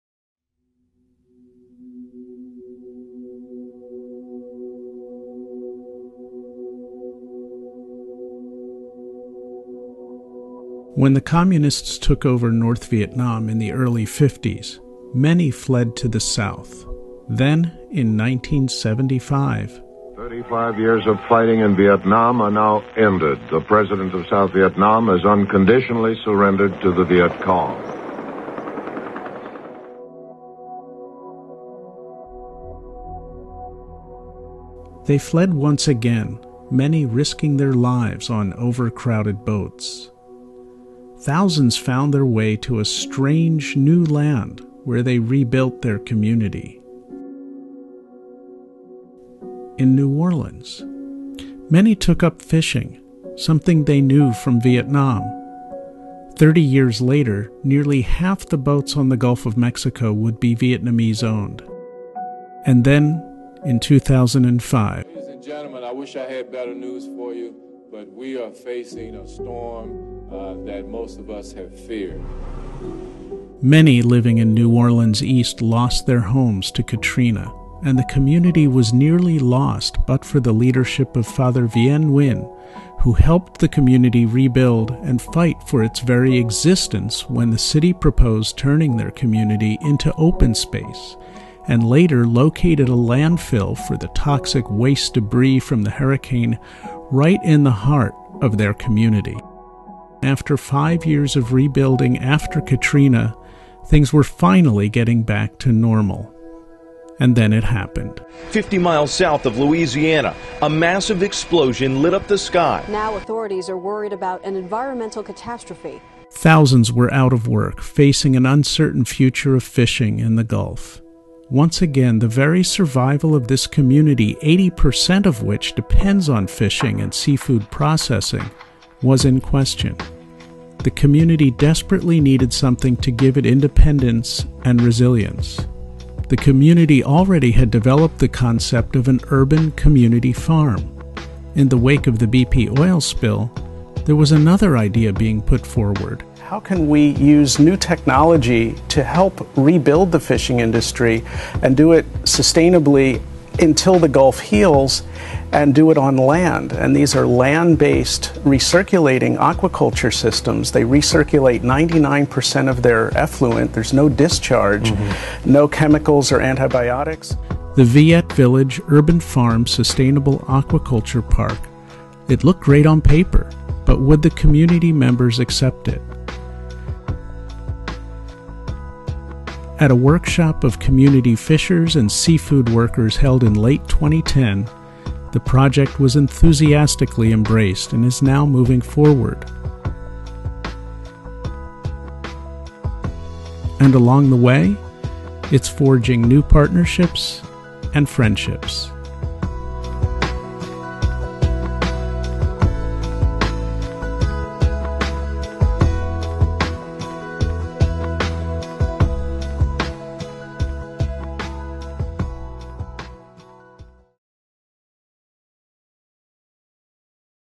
Viet Village Urban Farm: Creating a Sustainable Aquaculture Park
After recovering from Hurricane Katrina, the Vietnamese fishing community of East New Orleans was devastated by the Deepwater Horizon explosion. Determined to survive, the community has come together to create a sustainable aquaculture park and urban farm in Louisiana.